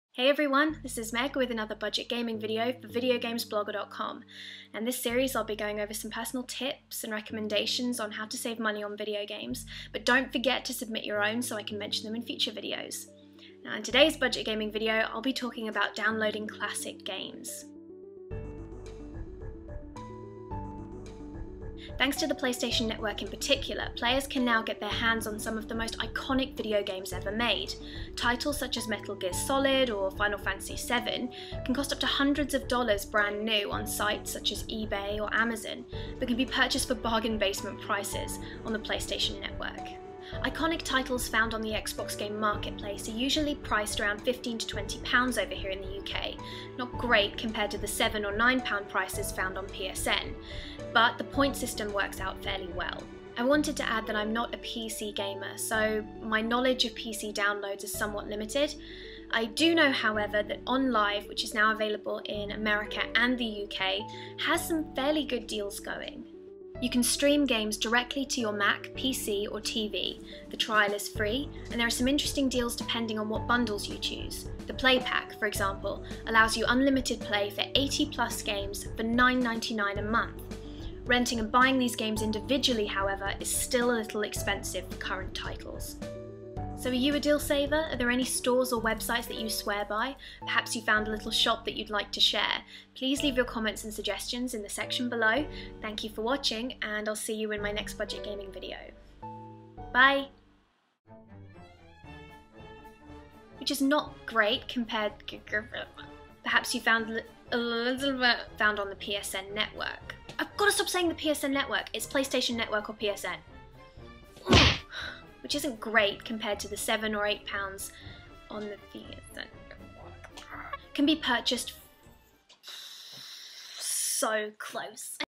Budget Gaming: Downloading the Classics!
Hello everyone and welcome to VGB's Budget Gaming feature with Meg!

In this video series I'll be going through my personal tips and recommendations for saving money on video games! But don't forget to submit your own, so I can feature them in future videos!

In this budget gaming video I'll be talking about renting video games!